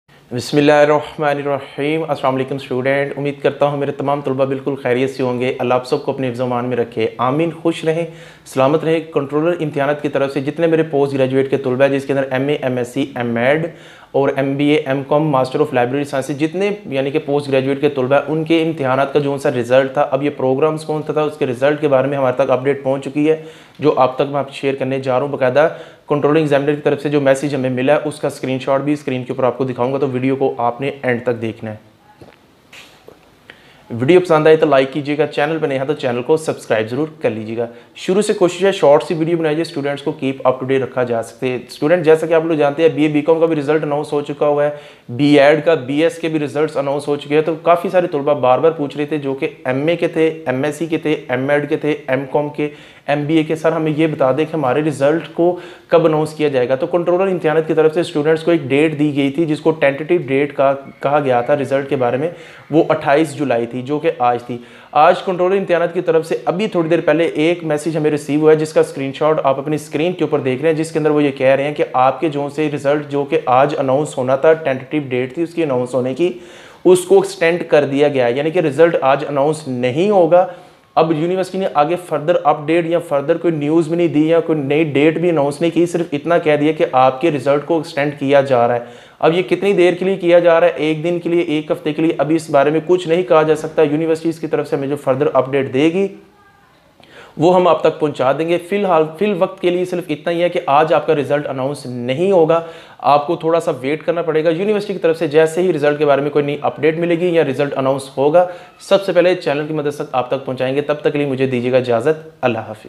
AIOU Post Graduates Result Autumn 2020 || AIOU News Update 2021
In this video, I will tell you about the latest announcement for all the students of AIOU and daily AIOU Updates 2021 and AIOU Information 2021.

About: Commerce Therapy is a YouTube Channel, where you will Online Lectures/AIOU Updates, New Video is Posted Everyday :)